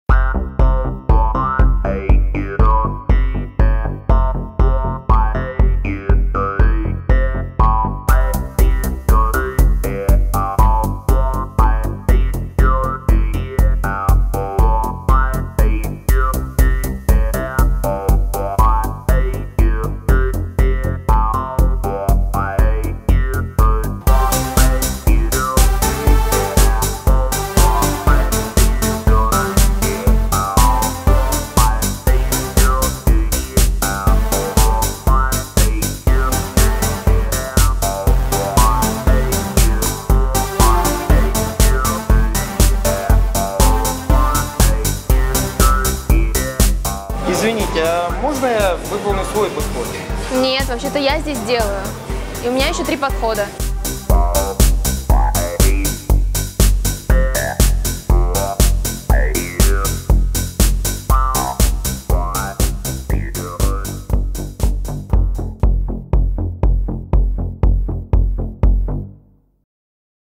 Правила поведения в фитнес клубе. №5. Не занимай чужие тренажеры. Никогда не делай этого в зале!
Никогда не занимай чужие тренажеры! Не беда Индивидуальные уроки с Юрием Спасокукоцким через Skype или Vabber! Правила поведения в фитнес клубе. Никогда не делай этого в тренажерном зале!
Продолжаю снимать и публиковать на своем канале видео из цикла "Никогда не делай этого в тренажерном зале!". Сегодня ролик о такой не совсем хорошей ситуации когда твой тренажер занимает другой человек.

Наверное вам часто доводилось лично с таким встречаться, возможно и неоднократно. Хорошо если все происходит корректно, когда тебя спрашивают разрешения и вы вдвоем, по-очереди, работаете на тренажере. Но не всегда это бывает именно так.
Поэтому, будьте вежливыми в зале, не злоупотребляйте терпением других людей. Прежде чем начинать заниматься на тренажере, внимательно посмотрите по сторонам, может на нем кто-то уже работает.
Получи по настоящему эффективную программу питания и тренировок от профессионала Тренировки с Юрием в Москве